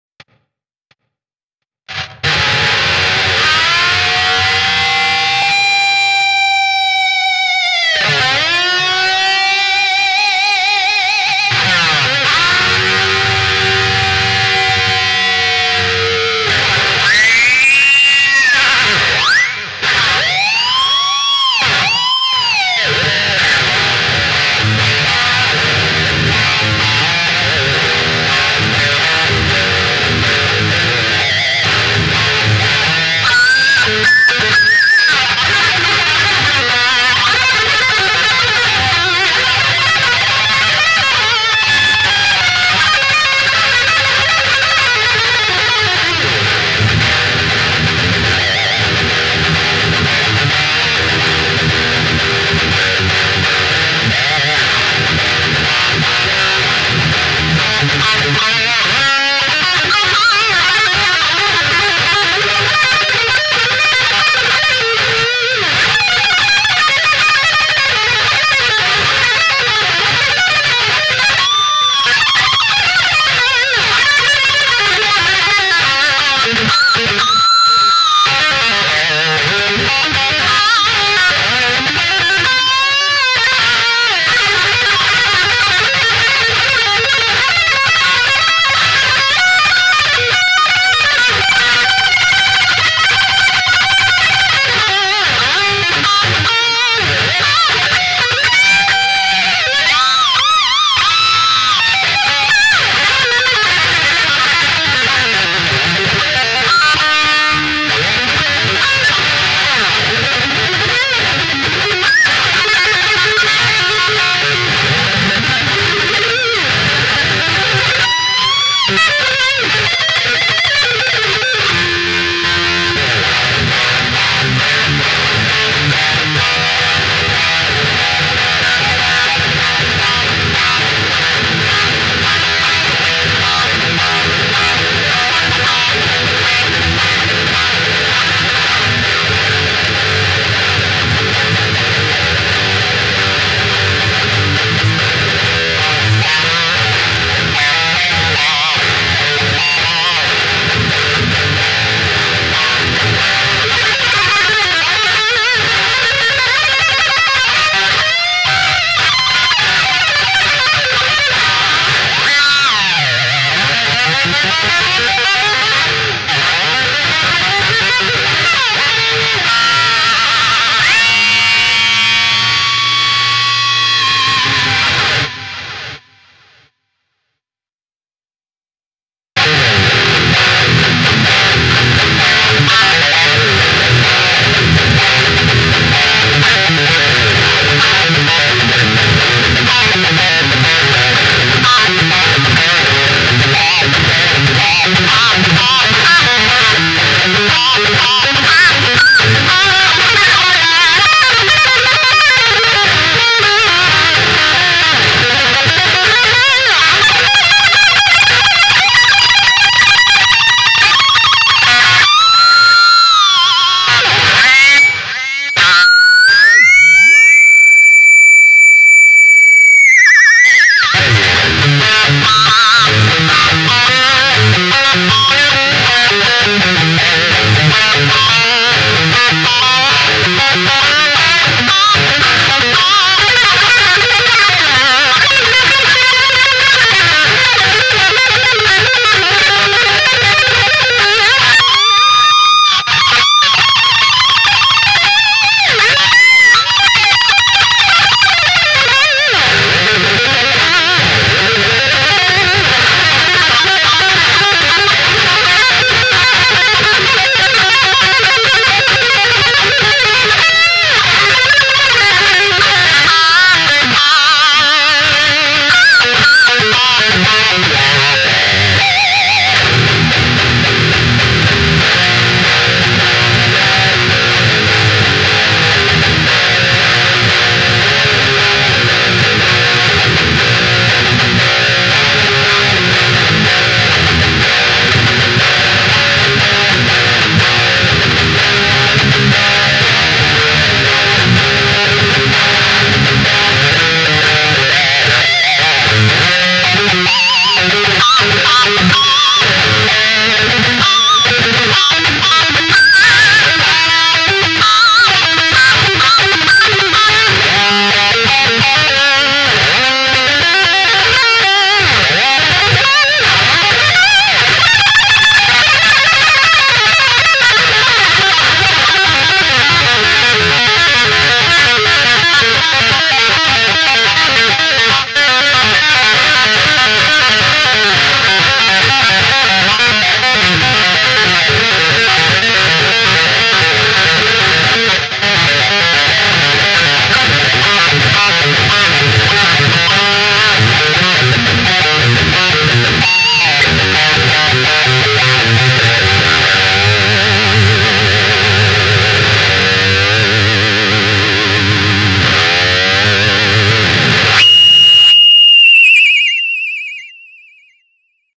Metallicaster
Just random shredding played in my own style